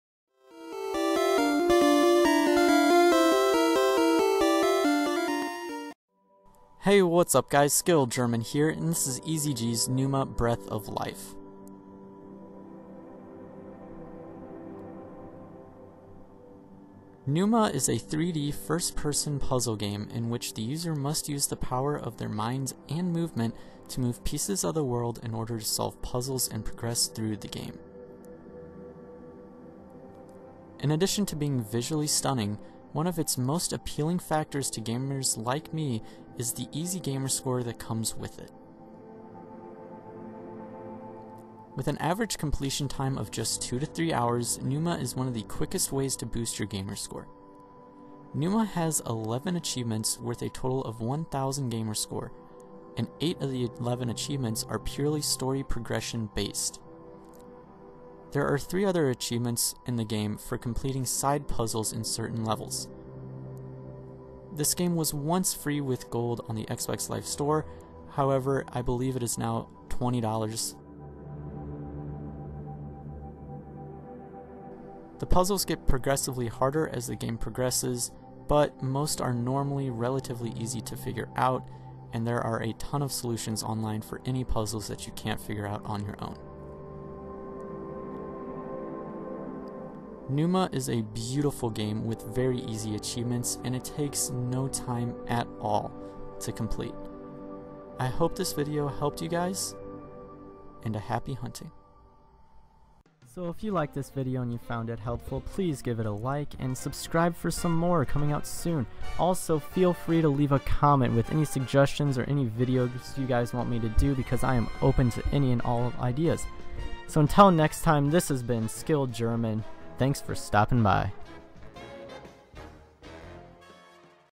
Easy G's - Pneuma: Breath of Life
Skilled German overviews Pneuma: Breath of Life on the first edition of Easy G's

Follow on Twitter @SkilledGerman